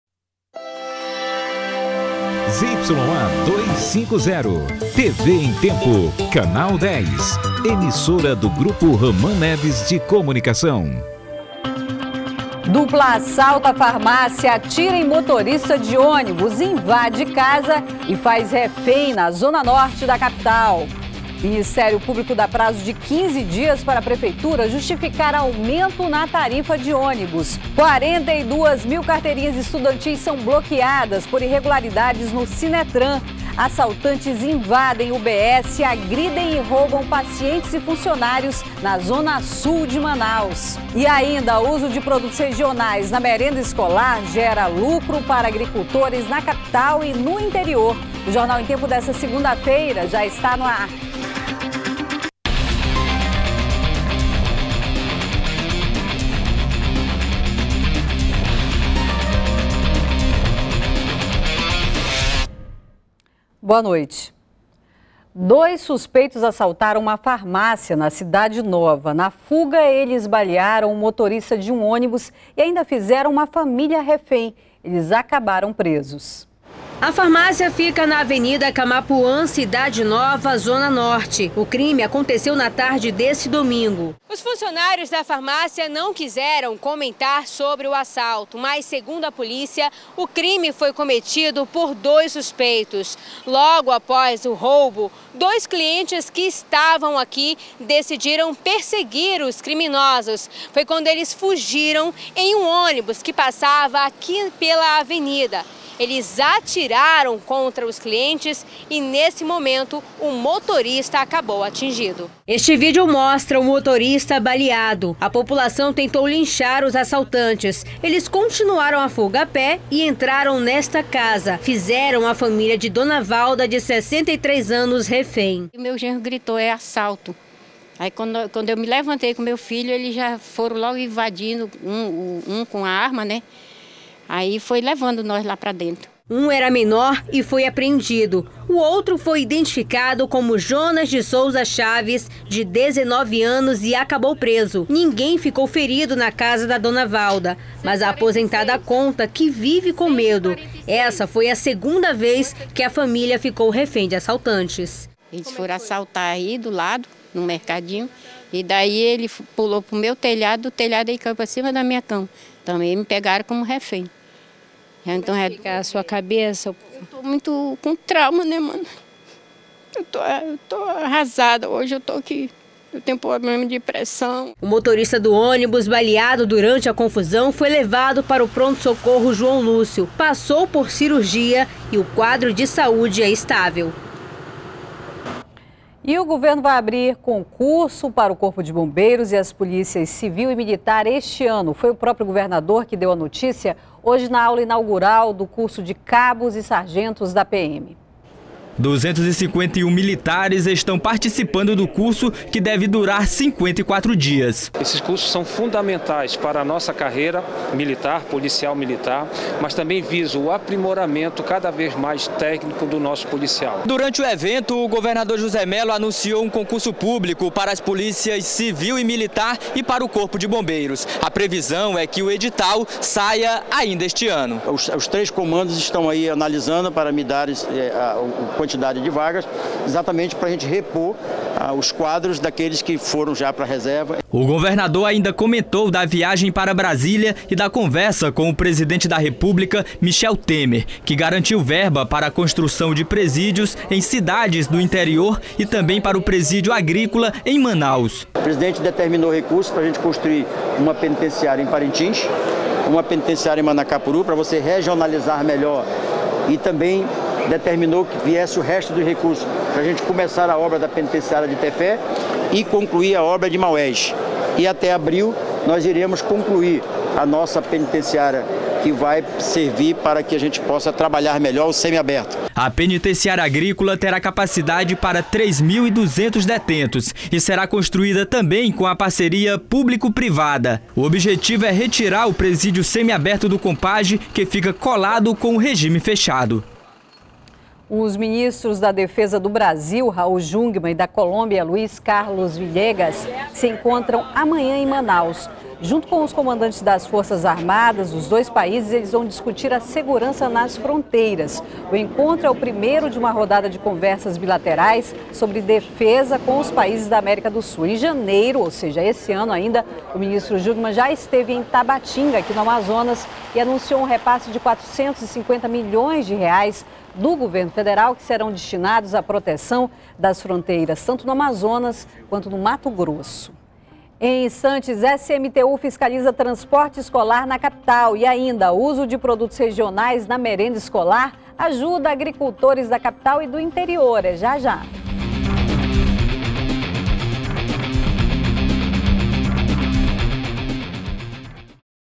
Presa a Dupla que assaltou farmácia, baleou um motorista de ônibus e fez uma idosa refém
01º BLOCO JORNAL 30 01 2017 - Presa a Dupla que assaltou farmácia, baleou um motorista de ônibus e fez uma idosa refém